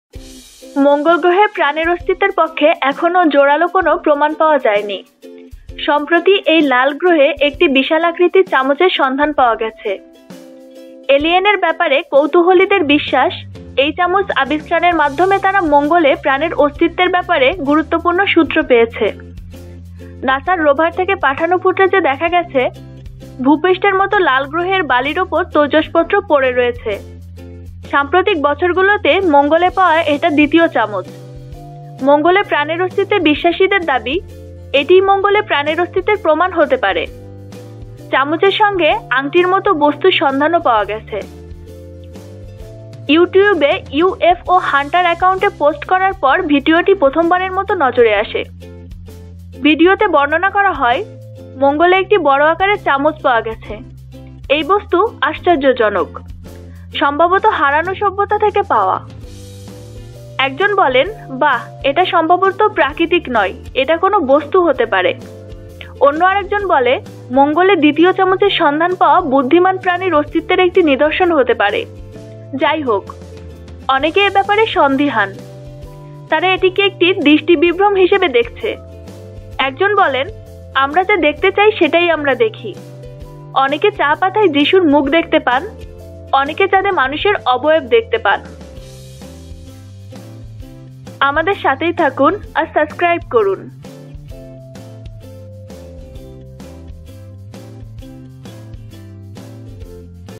মঙ্গলে বিশালাকৃতির চামচ !
মঙ্গল গ্রহে প্রাণের অস্তিত্বের পক্ষে এখনো জোরালো কোনো প্রমাণ পাওয়া যায়নি। সম্প্রতি এই লাল গ্রহে একটি বিশাল আকৃতির চামচের সন্ধান পাওয়া গেছে। এলিয়েনের (ভিনগ্রহবাসী) ব্যাপারে কৌতূহলীদের বিশ্বাস, এই চামচ আবিষ্কারের মাধ্যমে তারা মঙ্গলে প্রাণের অস্তিত্বের ব্যাপারে গুরুত্বপূর্ণ সূত্র পেয়েছে।

নাসার রোভার থেকে পাঠানো ফুটেজে দেখা গেছে, ভূপৃষ্ঠের মতো লাল গ্রহের বালির ওপর তৈজসপত্র পড়ে রয়েছে। সাম্প্রতিক বছরগুলোতে মঙ্গলে পাওয়া এটা দ্বিতীয় 'চামচ'। মঙ্গলে প্রাণের অস্তিত্বে বিশ্বাসীদের দাবি, এটিই মঙ্গলে প্রাণের অস্তিত্বের প্রমাণ হতে পারে।

চামচের সঙ্গে আংটি ও দস্তানার মতো বস্তুর সন্ধান পাওয়া গেছে। ইউটিউবে ইউএফও হান্টার অ্যাকাউন্টে পোস্ট করার পর ভিডিওটি প্রথমবারের মতো নজরে আসে। ভিডিওতে বর্ণনা করা হয়, ‘মঙ্গলে একটি বড় আকারের চামচ পাওয়া গেছে! এই বস্তু আশ্চর্যজনক! সম্ভবত হারানো সভ্যতা থেকে পাওয়া।’

কয়েকজন ইউটিউব ব্যবহারকারী এই আবিষ্কারে বিস্ময় প্রকাশ করেছেন। একজন বললেন, ‘বাহ, এটি সম্ভবত  প্রাকৃতিক নয়, এটা কোনো বস্তু হতে পারে।’

অন্য আরেকজন বলেন, ‘মঙ্গলে দ্বিতীয় চামচের সন্ধান পাওয়া বুদ্ধিমান প্রাণীর অস্তিত্বের একটি নিদর্শন হতে পারে।’

যাই হোক, অনেকে এ ব্যাপারে সন্দিহান। তারা এটিকে একটি দৃষ্টিবিভ্রম হিসেবে দেখছে। একজন বলেন, ' আমরা যা দেখতে চাই সেটাই আমরা দেখি। অনেকে  চা-পাতায় যিশুর মুখ দেখতে পান। অনেকে চাঁদে মানুষের অবয়ব দেখতে পান।’